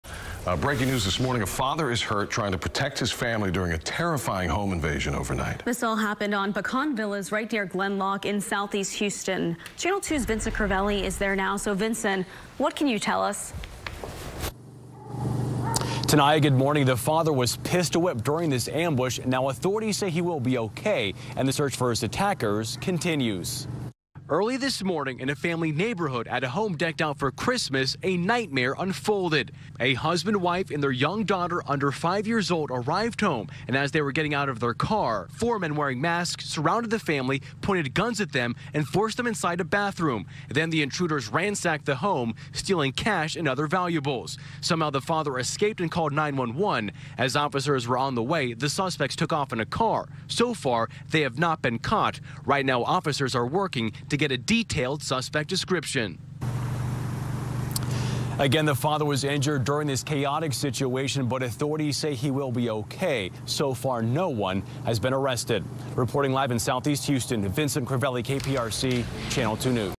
Man pistol-whipped, family held at gunpoint during southeast Houston home invasion
A man was pistol-whipped early Friday morning and his family was held at gunpoint during a home invasion in southeast Houston.